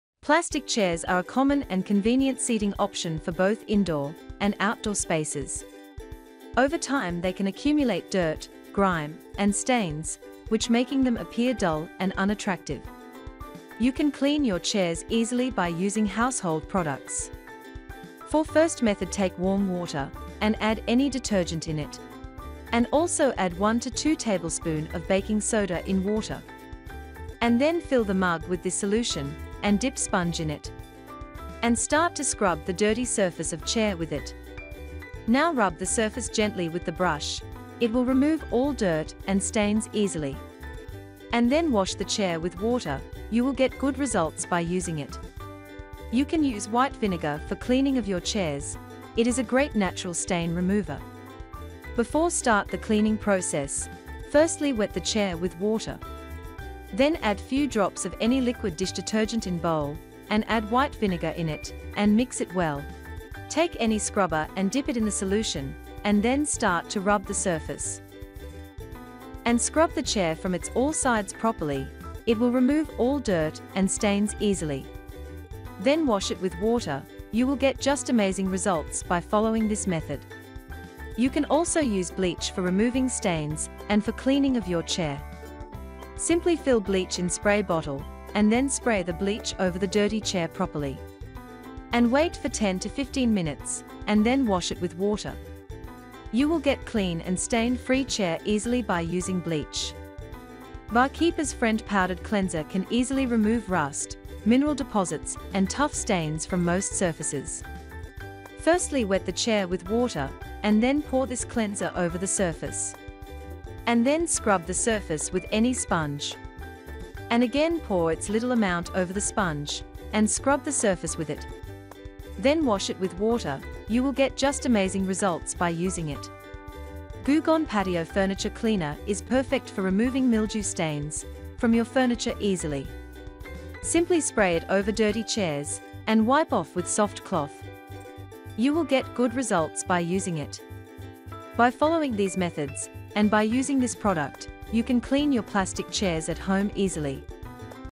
How To Clean Plastic Chairs At Home - Quick & Easy Cleaning Hacks
💥Welcome To Top To Bottom Cleaning Channel💥